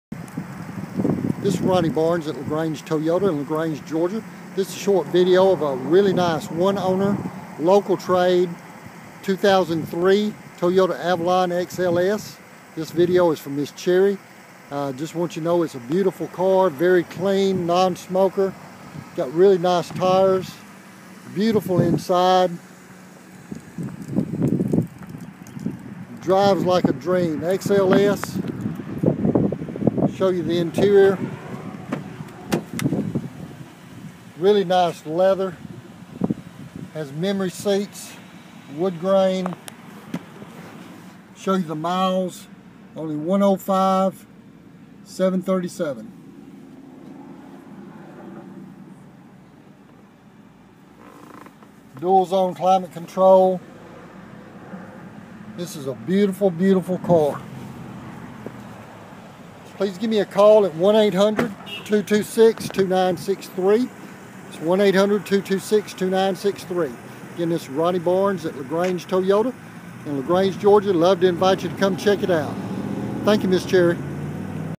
2003 Toyota Avalon XLS review by Ronnie Barnes
2003 Toyota Avalon XLS review by Ronnie Barnes at Lagrange Toyota